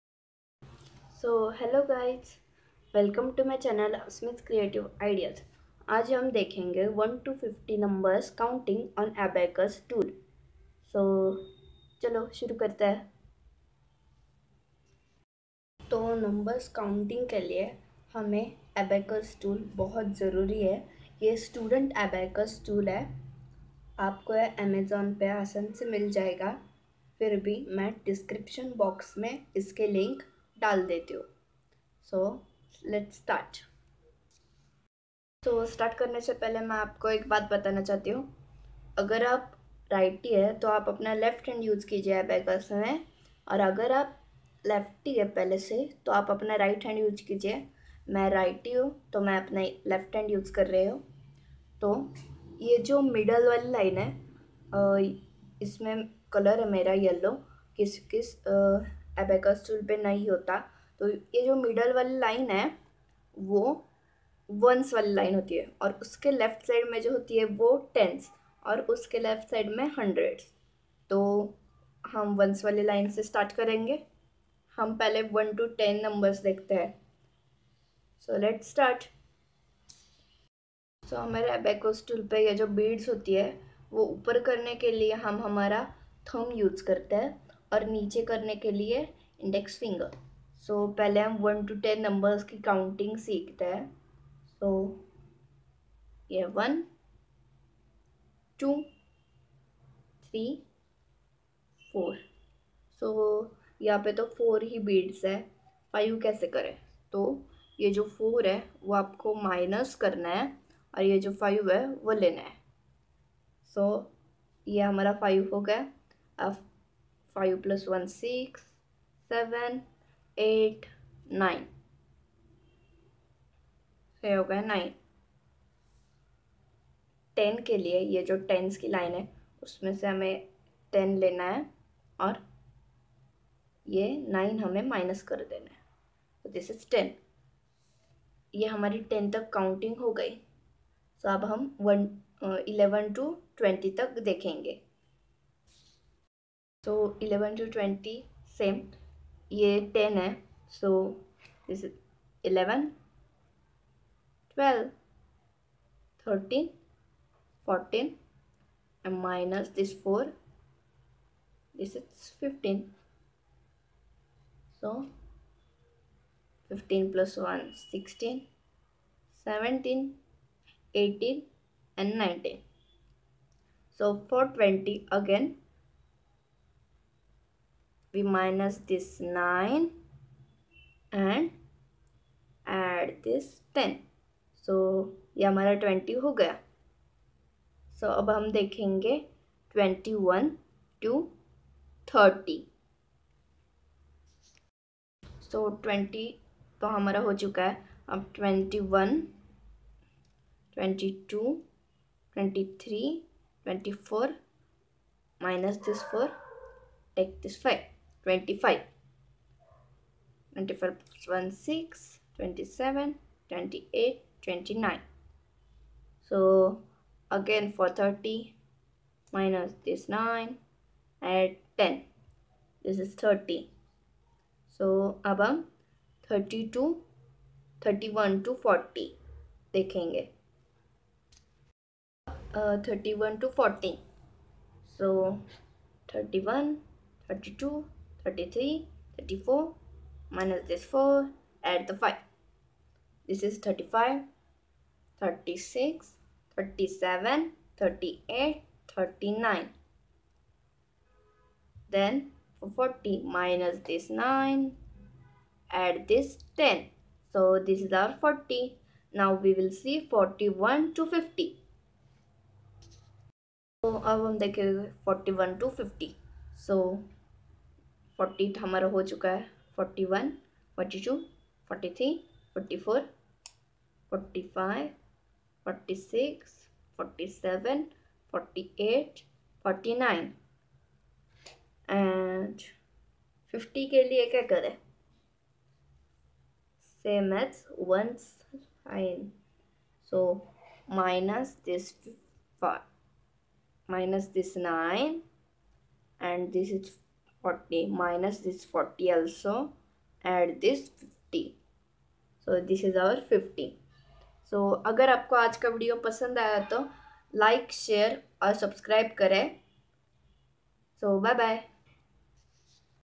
Counting numbers 1 to 50 on Abacus tool
Xtragenius 15 Rod Brown Color Abacus Tool(5 Unit)

Buy link for Abacus tool 👆